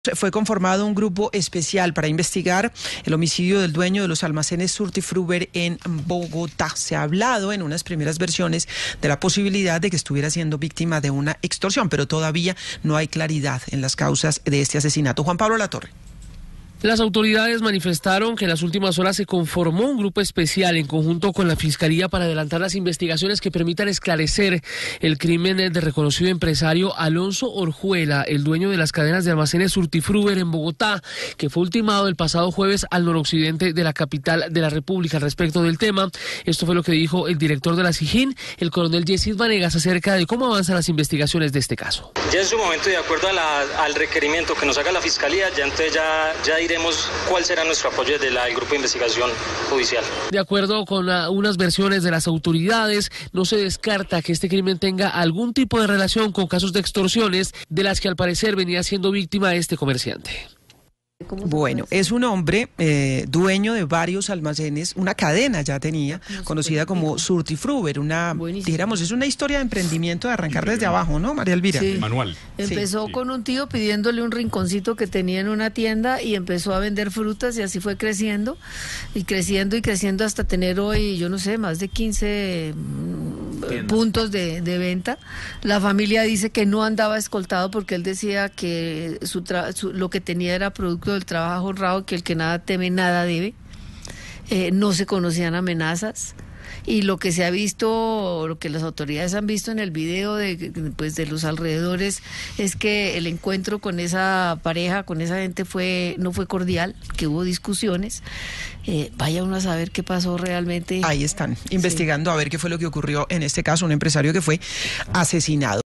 Crean grupo especial para esclarecer el crimen del empresario Alonso Orjuela
La Policía conformó un grupo especial para investigar el homicidio de Alonso Orjuela, el empresario propietario de la cadena de almacenes Surtifruver, que fue asesinado el pasado 20 de octubre en Bogotá.

Según se pudo conocer, la policía y la fiscalía crearon un equipo de investigación que permita esclarecer el crimen de Alonso Orjuela.

Investigaciones preliminares evalúan la posibilidad de que el crimen esté relacionado con un caso de extorción de la que al parecer era víctima el comerciante.